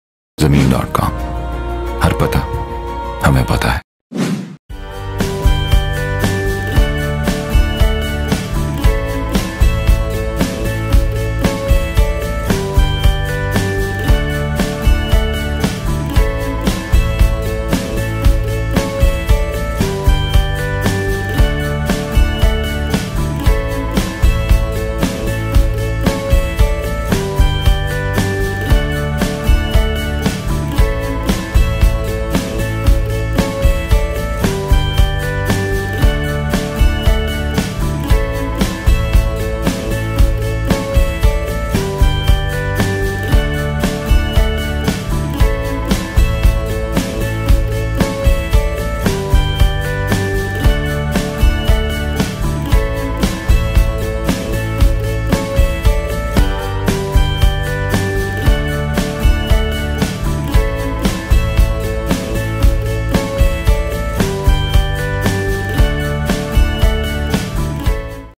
1 KANAL RESIDENTIAL PLOT FOR SALE IN SECTOR M-3 LAKE CITY LAHORE
1 Kanal Residential Plot For Sale Ready To Construct On Hot Location

Consult N Deal Lake City , Authorized Real Estate Company of The Society with Largest Professionally Trained Sales Consultant for Your Right Guidance, Offers . .

Project Lake City :

* Sector M3
* Plot No ***
* Developed Plot
* Excellent Location
* Ideal Location for Investment Opportunity.

Main Society Features Are:

* Around 2500 Families Living Happily
* No Load Shedding
* Under Ground Electrification
* 6 Megawatt Own Power Plant
* Security
* Sky Zone (Kids Play land)
* Lake City Mall ( Up Coming )
* Lush Green Golf Course
* Cinema
* Zoo
* Banks
* Pharmacy
* Mart
* International Standard Froebels School
* Ring Road Excess
* Prime Location.
* Reasonable Demand.